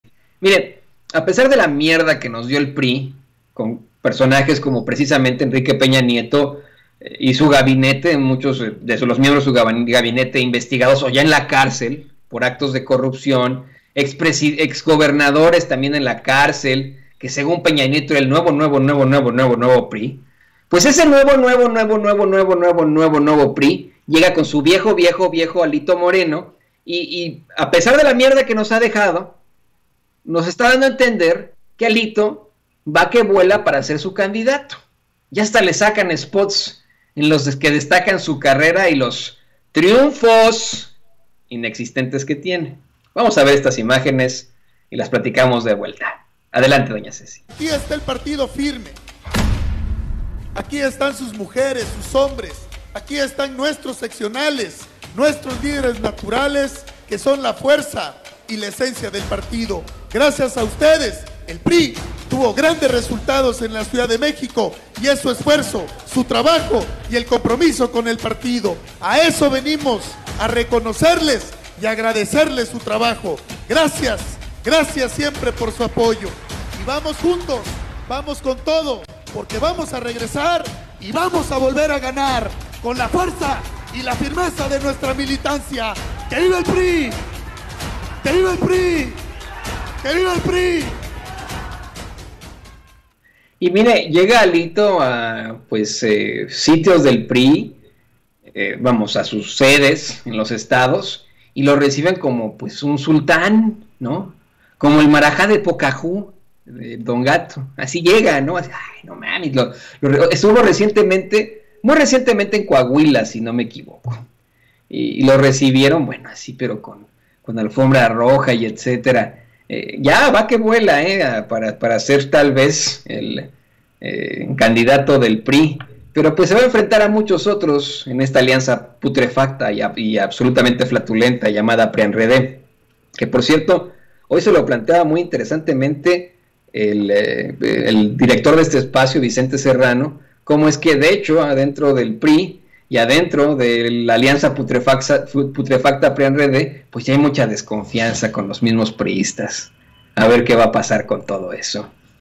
A PESAR DE LA MIERDA QUE NOS DIO EL PRI, ALITO VA QUE VUELA PARA SER SU CANDIDATO
Y a pesar de la mierda que nos dio el PRI, todo indica que su dirigente Alito Moreno va que vuela para ser su candidato… Ya hasta le sacan spots en los que destacan su carrera y “triunfos” inexistentes.